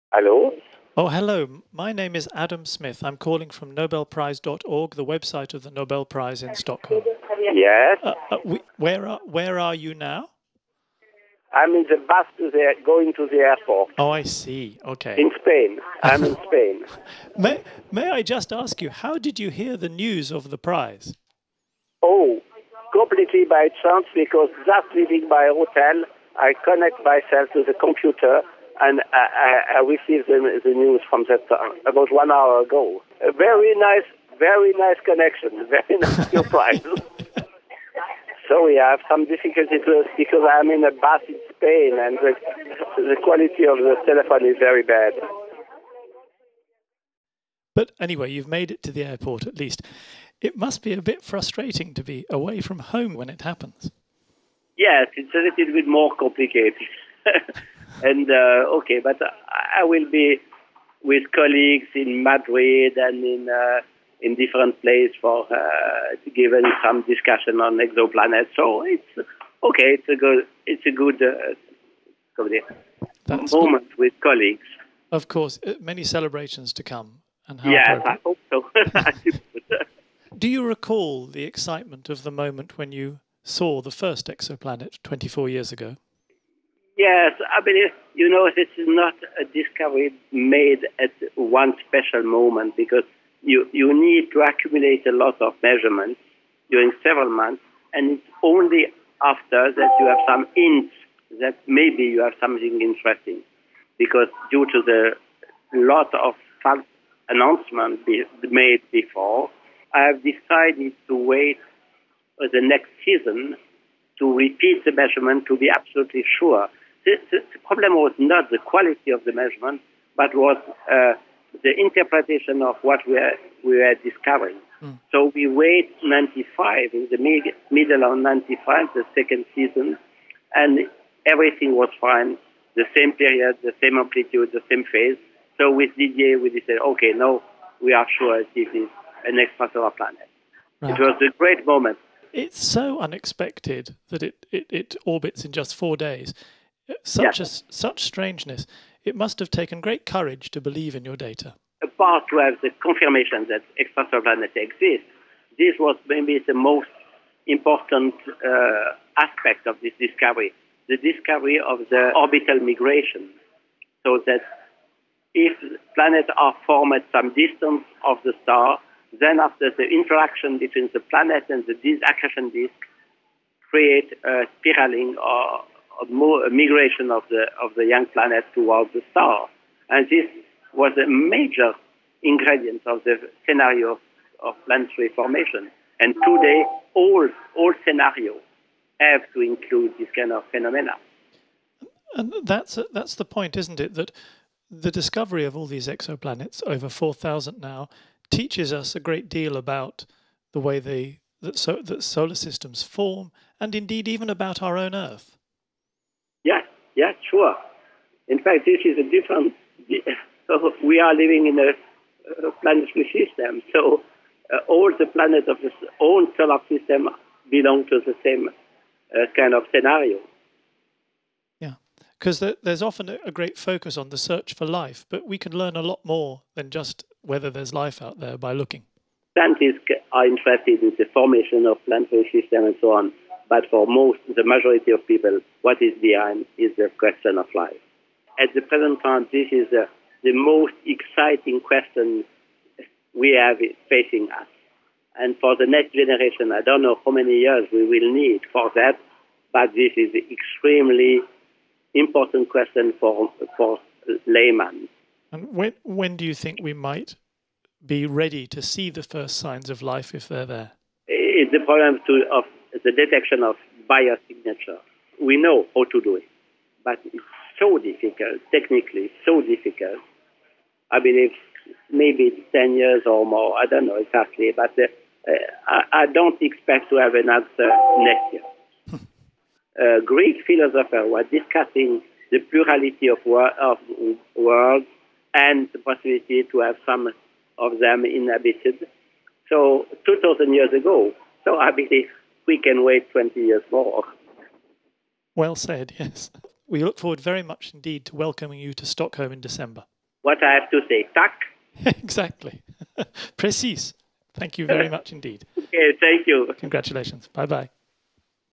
First reactions | Michel Mayor, Nobel Prize in Physics 2019 | Telephone interview
"Very nice connection, very nice surprise!” – Michel Mayor was in the midst of a lecture tour in Spain when he received the news of his Prize. In this conversation with Adam Smith, which starts on an airport bus and continues in the terminal of San Sebastian airport in Spain, he recalls how he heard about the prize, the care they took back in 1995 before releasing news of the first exoplanet to the world, and the ongoing search for life on other planets.